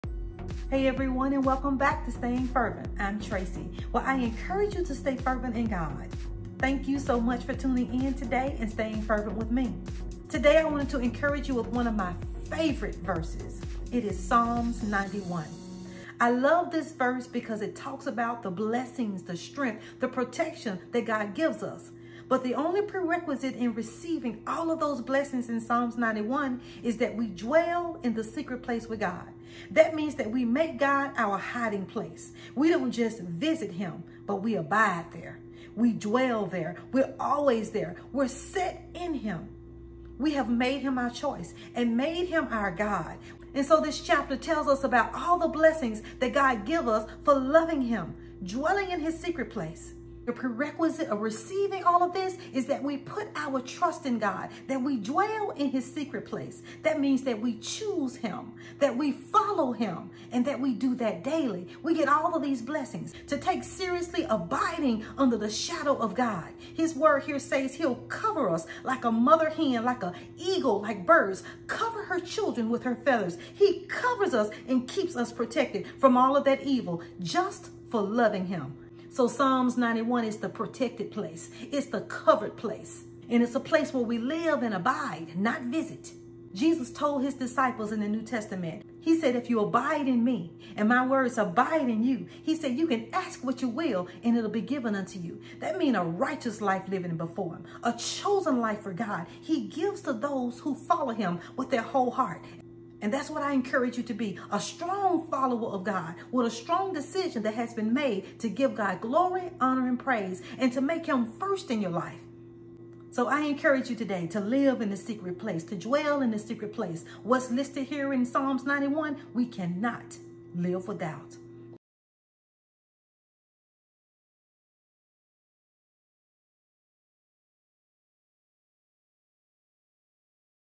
HOW GOD COVERS THOSE WHO DWELL IN HIM
Our faith in God as Protector, will carry us through rough and dark times.

Reference;
Psalms 91

Be blessed and encouraged,
Tracy